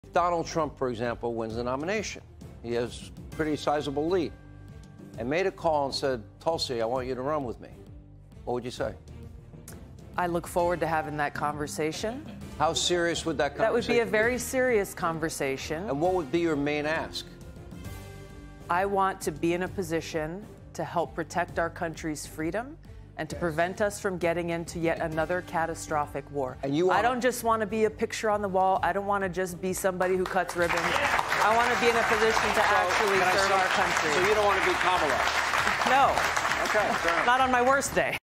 What would Tulsi do if Trump called?
Tulsi Gabbard answers Sean Hannity on whether she’d discuss being Trump’s running mate.